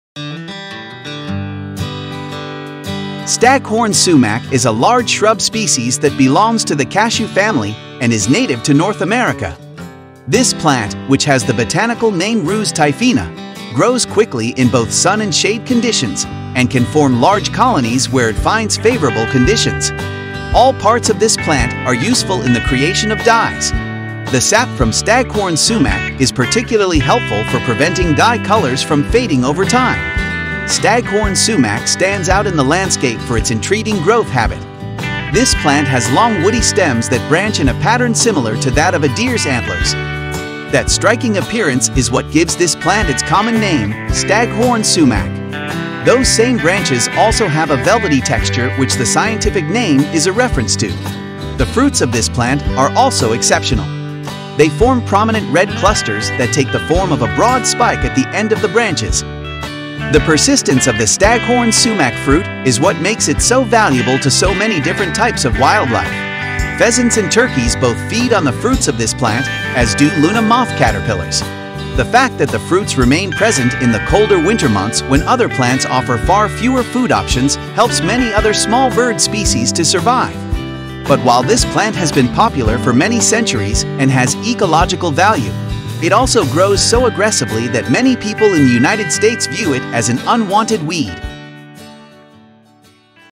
Love and compassion, rebirth--Staghorn sumac
The dried bobs of the Staghorn sumac are occasionally used by beekeepers for their smokers. With the exception of the roots, all parts of the plant can be used to make dyes or dye mordants; the tannins in the sap help prevent the dye colors from fading. Luna moth caterpillars feed on Staghorn sumac and the fruits are picked at by pheasants and wild turkeys.